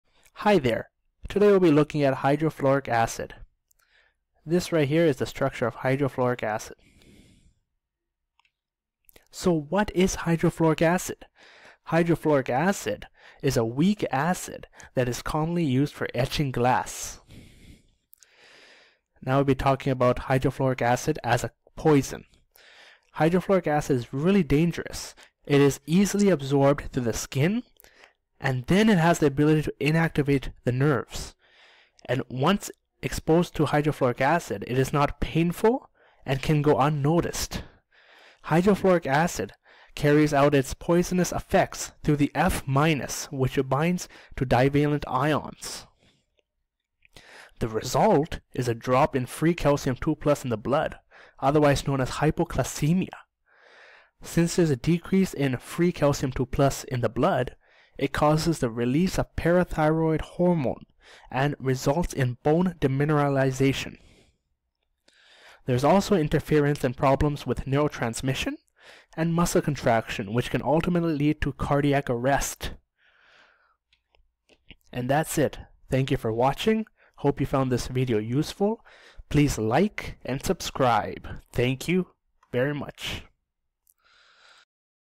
Hydrofluoric acid: A metabolic poison
This video explains Hydrofluoric acid as a metabolic poison.
HF;  metabolism; metabolic poison; calcium
mcat ; usmle step 1; mccee ; usmle ; medicine; medical ; premed